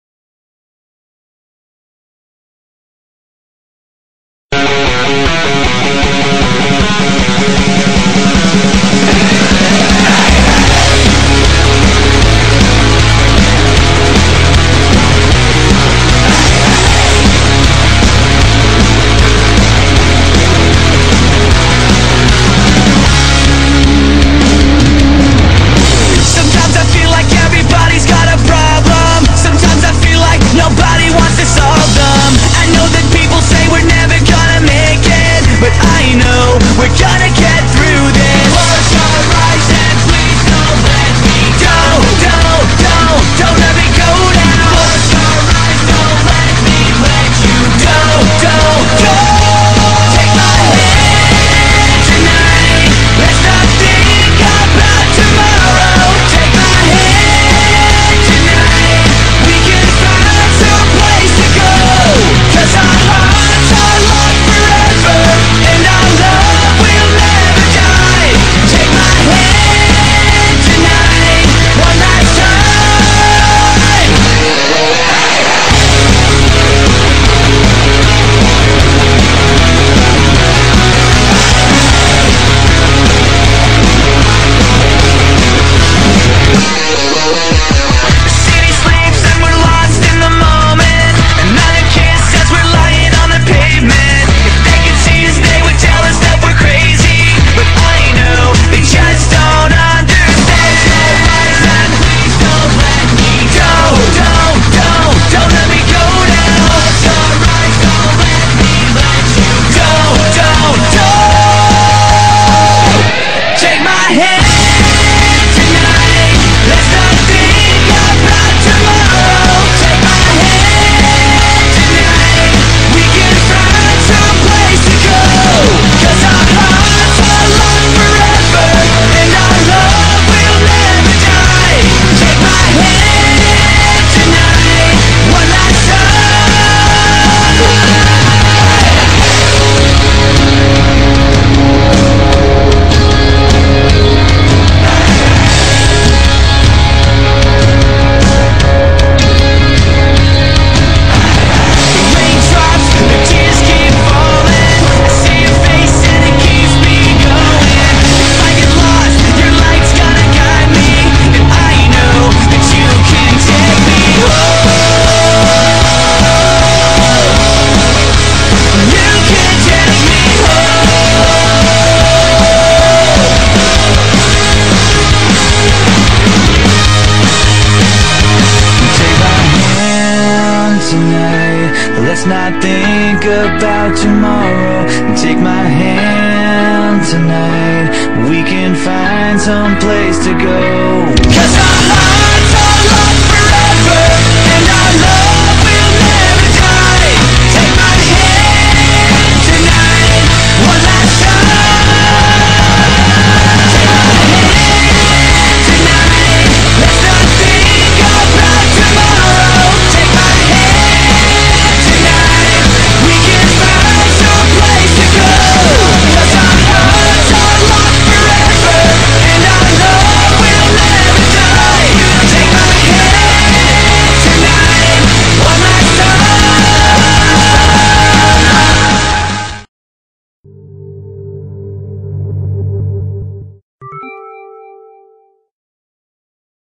take my hand jacob
another jacob and bella video hope u like it , dont hate me casue its not edward